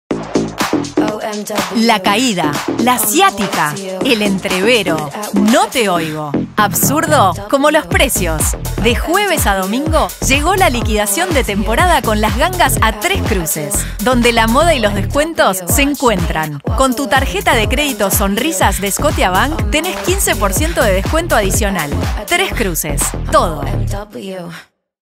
Gangas en Tres Cruces
Desde este jueves 08, hasta el domingo 11 aprovechá las GANGAS: 4 días con un montón de descuentos y precios increíbles!

Para conocer los locales adheridos, visitá nuestro sitio web